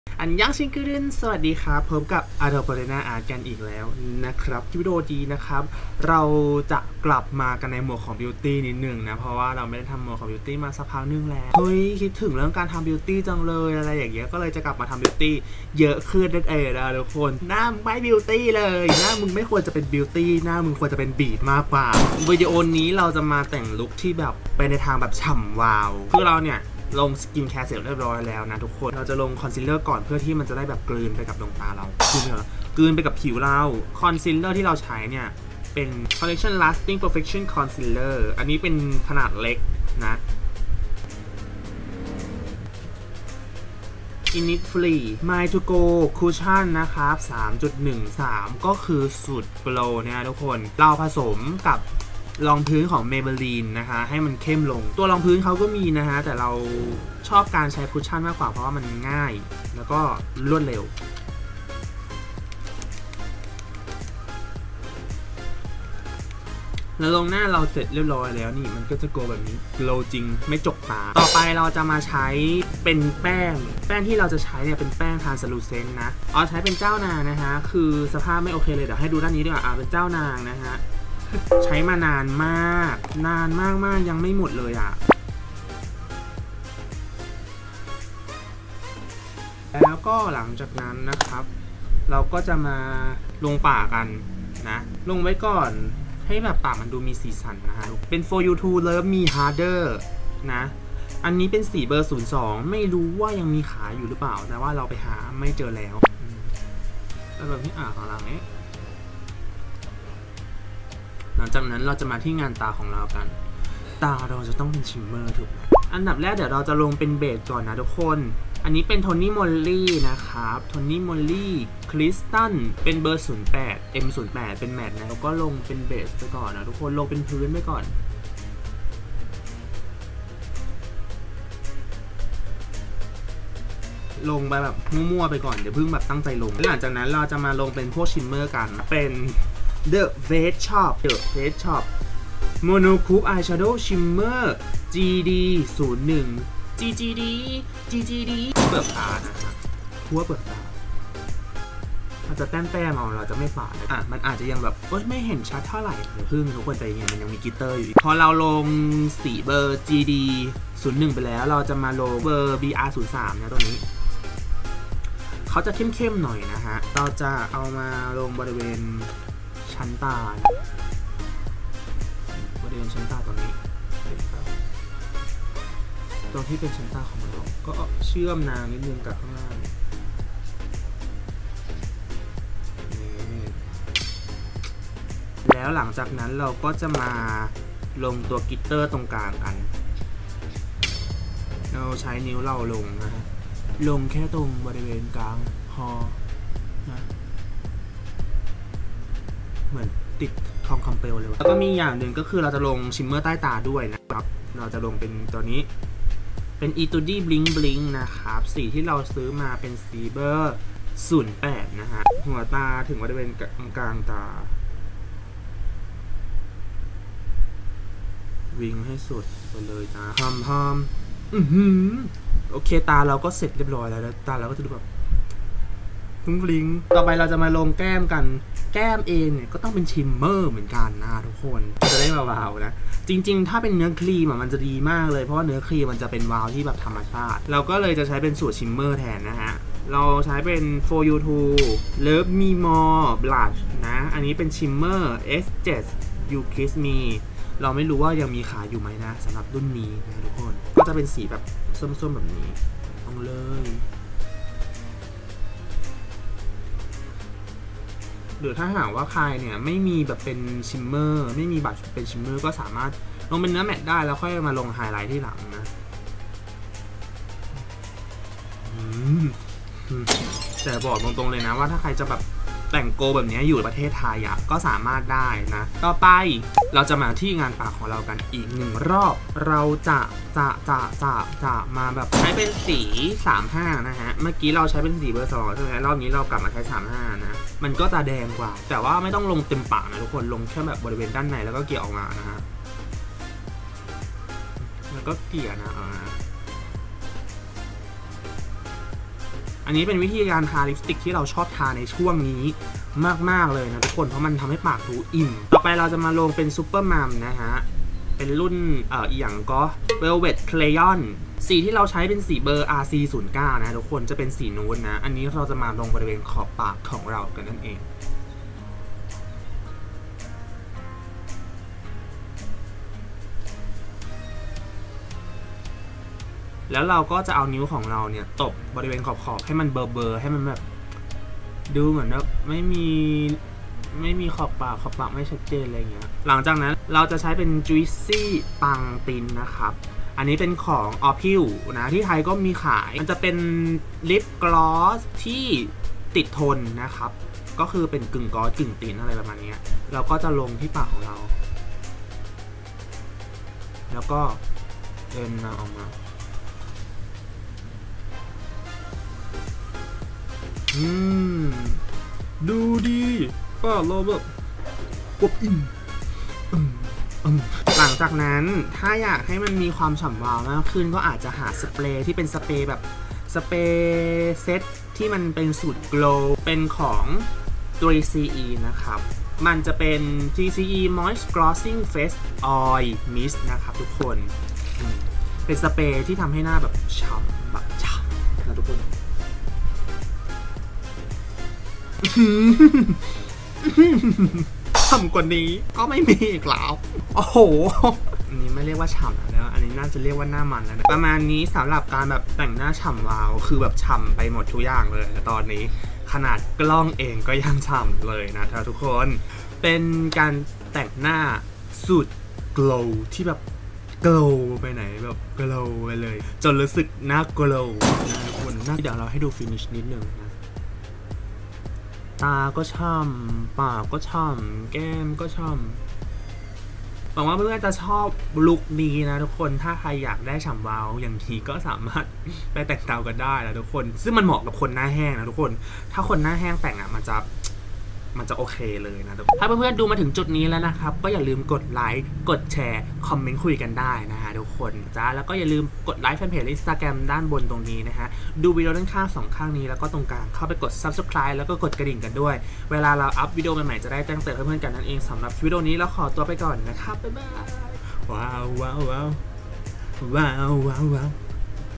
✨How to 62☀️ | หน้าฉ่ำกว่านี้ก็คือหน้ามันแล้ว | Arthopotena'Art
คลิปนี้มาแต่งหน้าแบบออลโกลว ออลฉ่ำ บอกเลยว่าถ้าไปออกแดด หน้ากับพระอาทิตย์ก็จะทักทายกัน